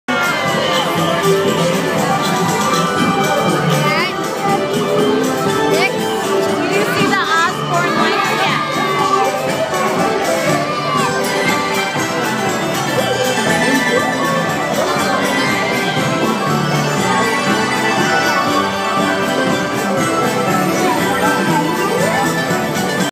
11/06/2015 Magic Kingdom Main Street Electrical Parade (Clip 3 of 5)
How many of you miss this spectacle? It was truly an amazing parade. I really hope it returns to Magic Kingdom in WDW.